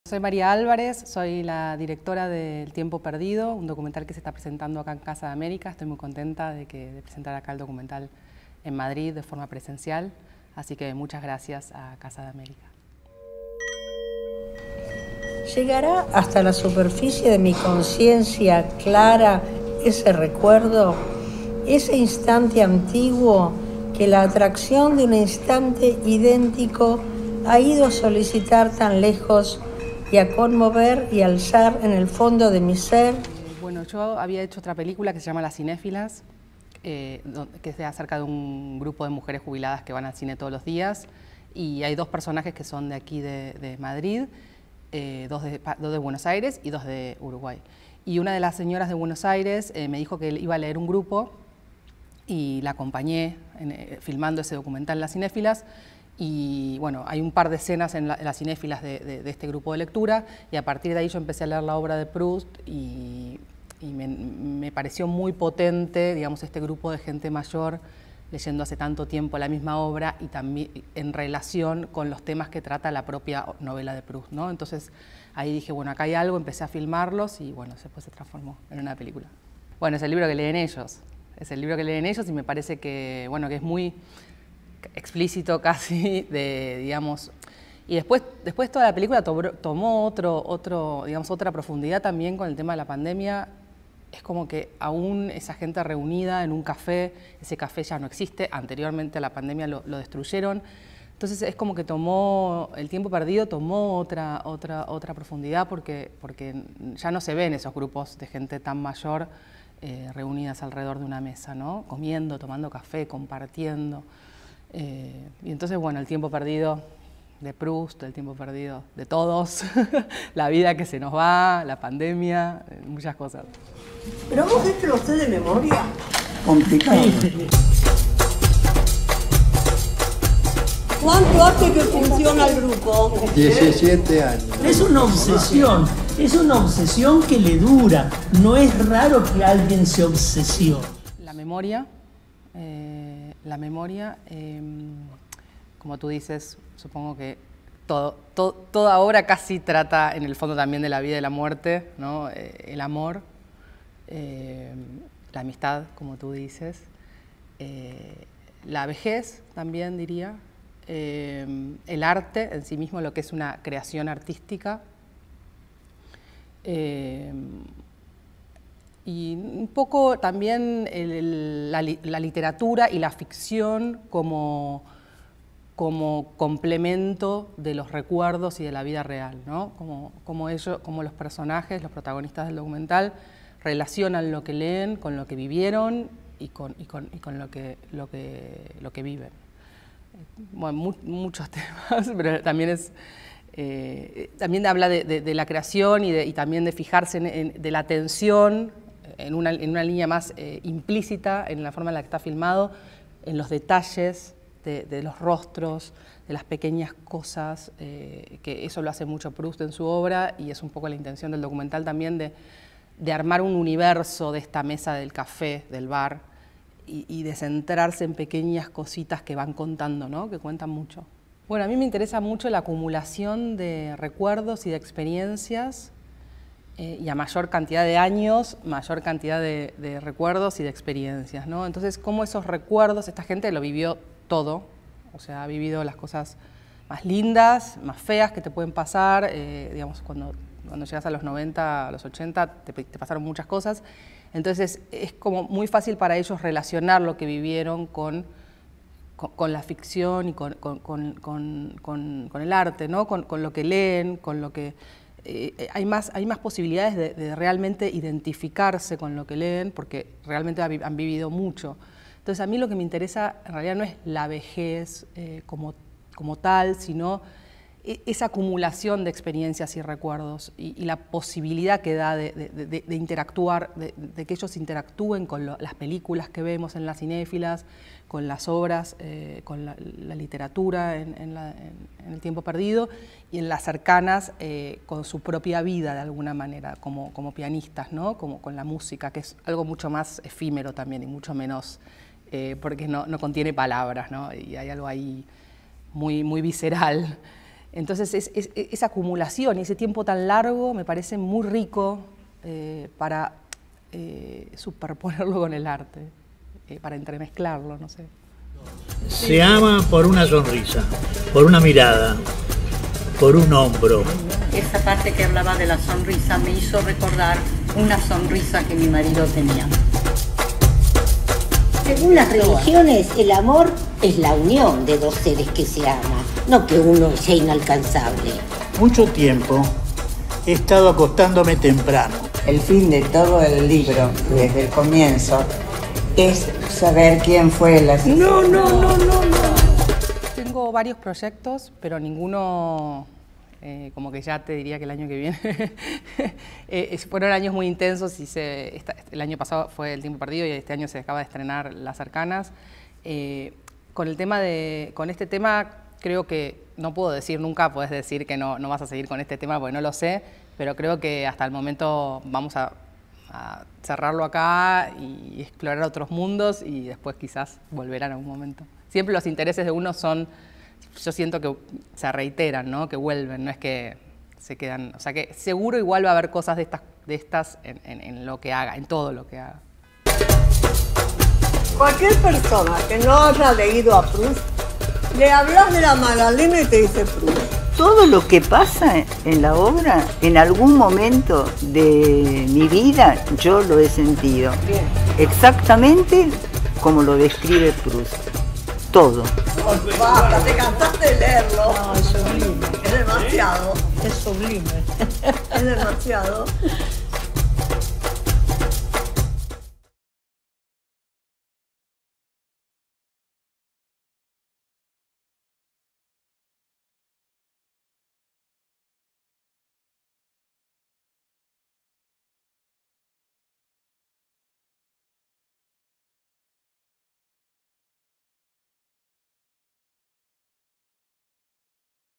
María Álvarez - El tiempo perdido
Entrevista a la directora del documental 'El tiempo perdido'.